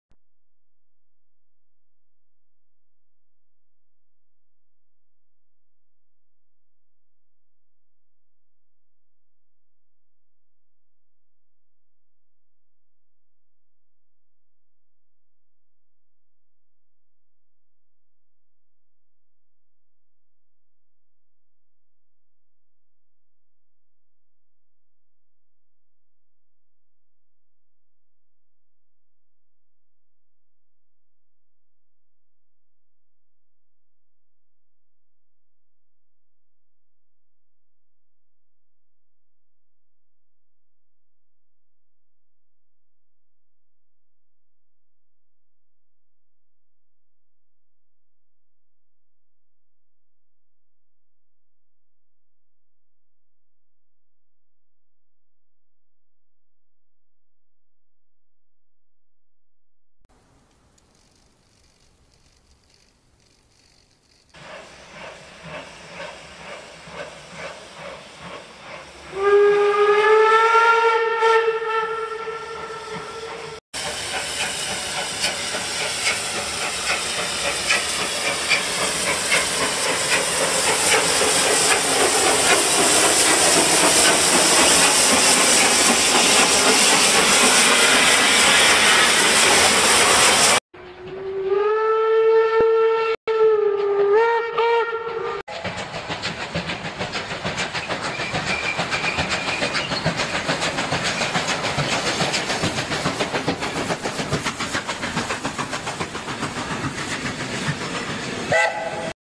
T-Gauge: Steam Locomotive 18 201 in Scale 1:480 (QTT) Is Now Finish
Sieben Jahre nach Baubeginn ist die 18 201 nun vollendet. In Spur T muss man einfach nur Geduld haben.
Sieht man das Video auf einem Display mit den Maßen 8x4,5cm, hat die Lok etwa Originalgröße.

Seven years after construction began, the 18 201 has now been completed. In T gauge you must have to be patient.
If you see the video on a display with the dimensions 8x4.5cm, the locomotive has about the original size.